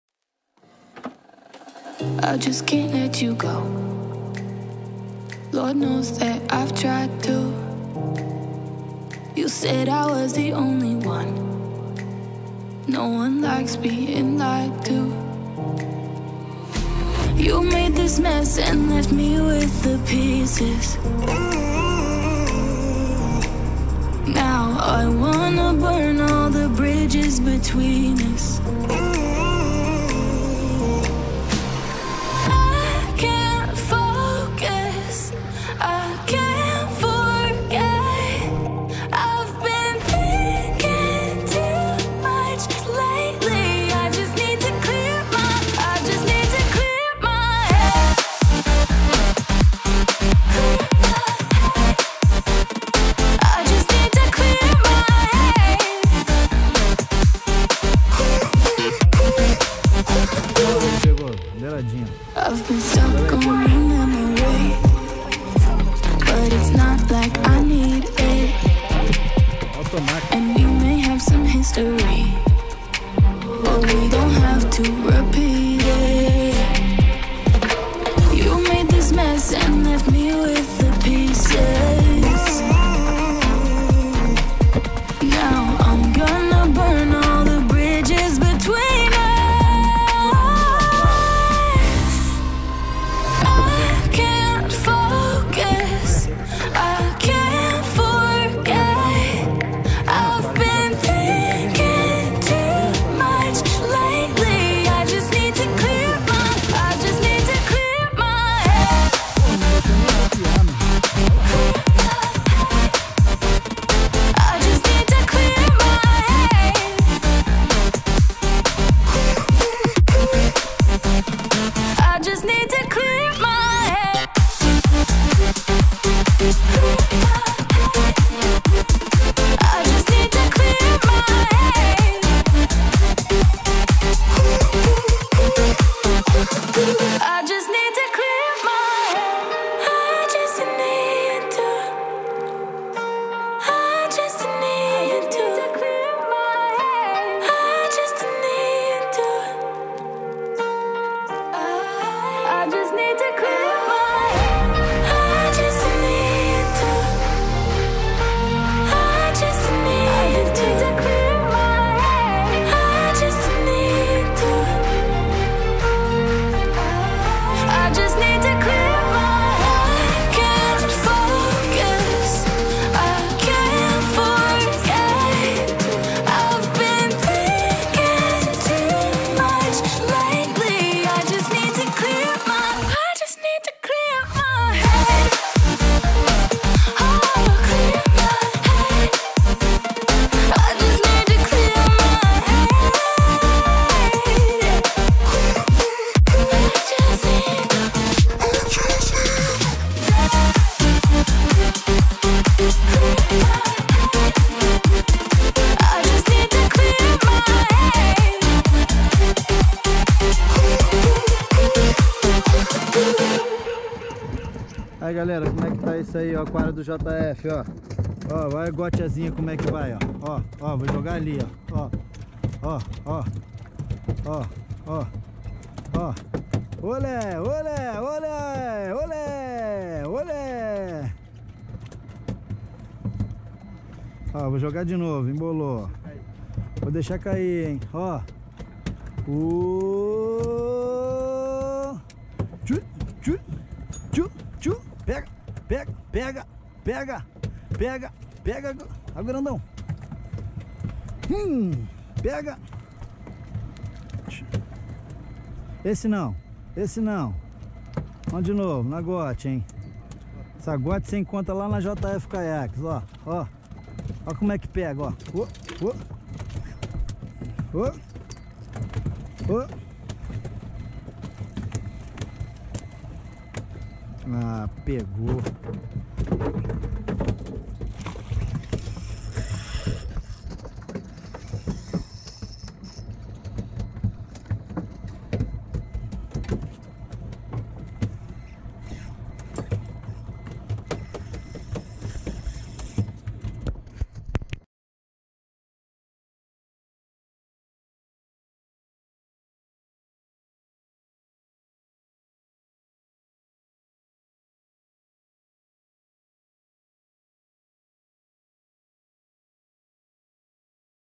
Quebradeira de Dourados - JF Caiaques e Pesca
Domingo de sol a Equipe da loja JF Caiaques e Pesca foi para o mar e fez essa linda pescaria de Dourado / Palombetas .
Quer saber mais sobre essa pescaria ?

Nós temos todo o material para essa pescaria
Rua Apiacás 535 lj B - Centro da Taquara RJ .